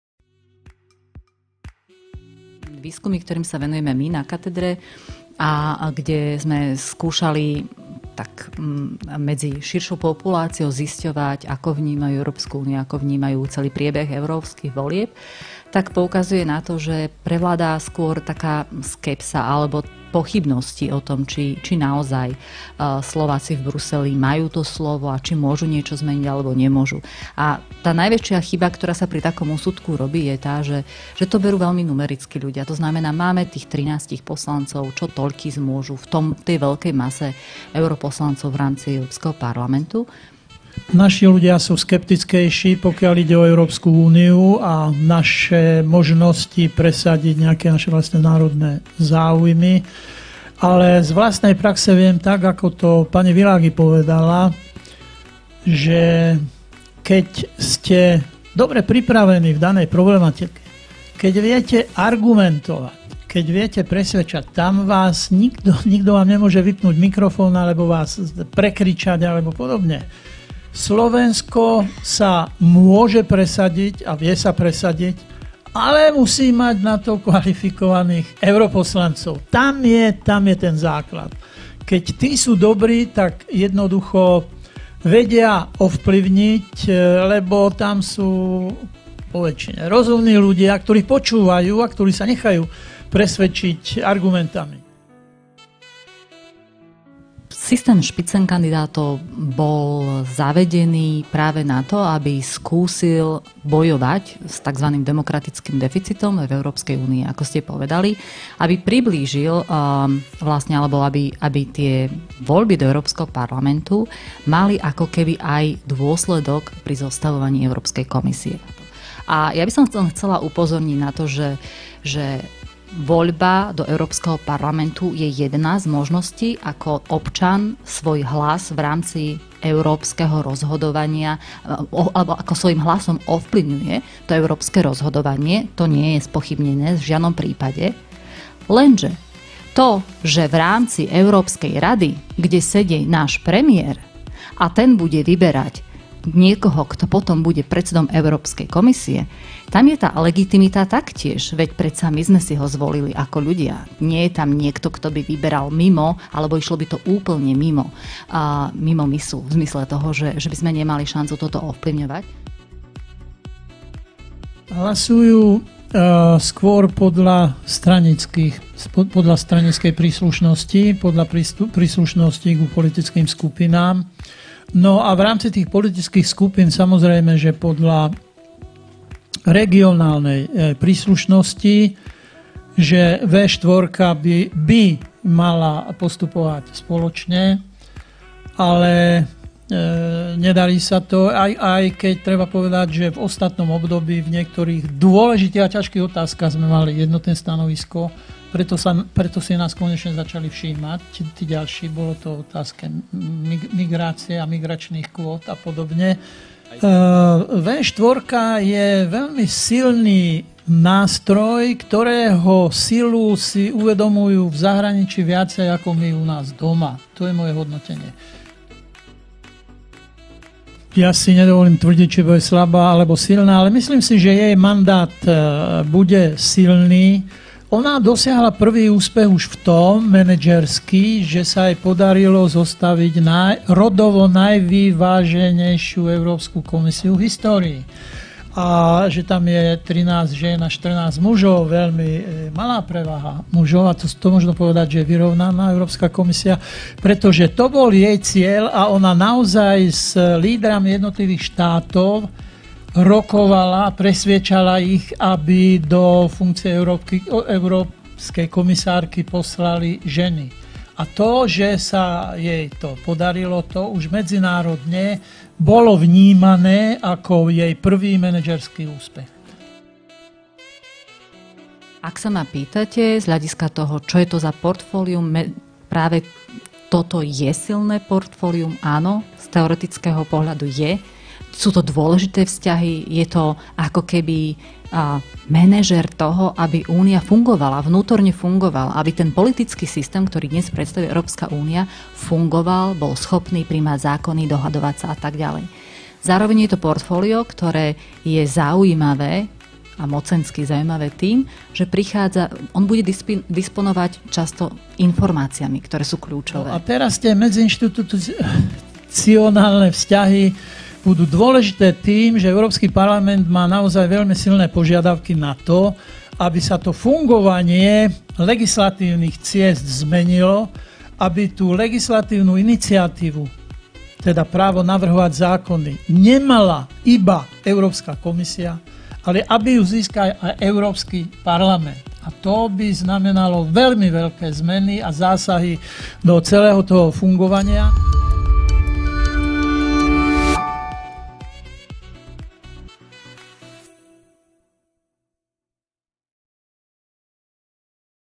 ZOSTRIH | Boj o moc v Bruseli
Dokáže Slovensko meniť rozhodnutia v Bruseli? Aké portfólio dostal Maroš Šefčovič na starosti a priblížili spitzenkandidáti Úniu občanom? Aj na tieto odpovede nájdete odpoveď v zostrihu z diskusie